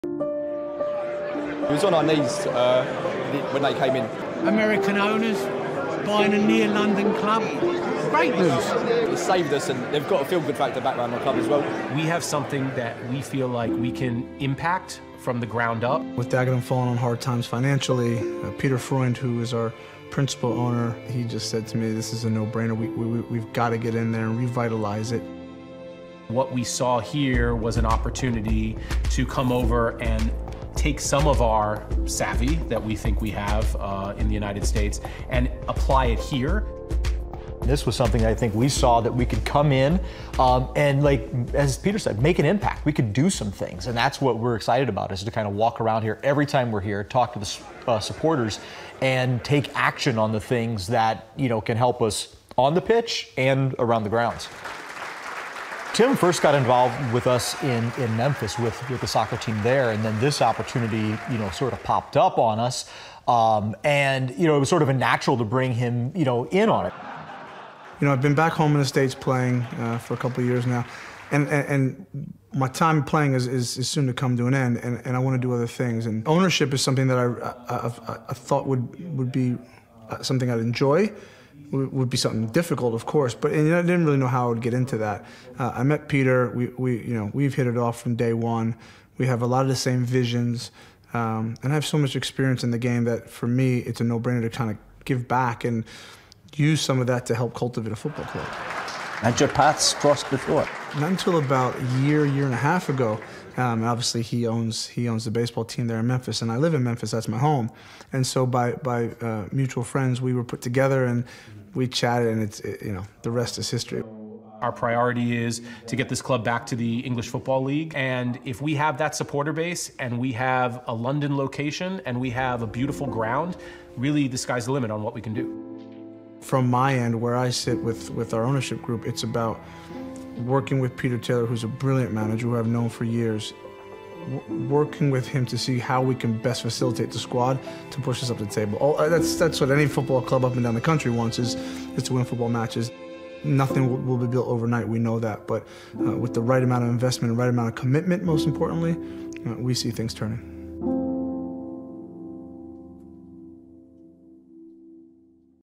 Why is Tim Howard investing in Dagenham & Redbridge?
From Premier League to National League. Former Everton and Manchester United goalkeeper Tim Howard explains how he became part of the consortium taking control of Dagenham & Redbridge, and his reasons behind it.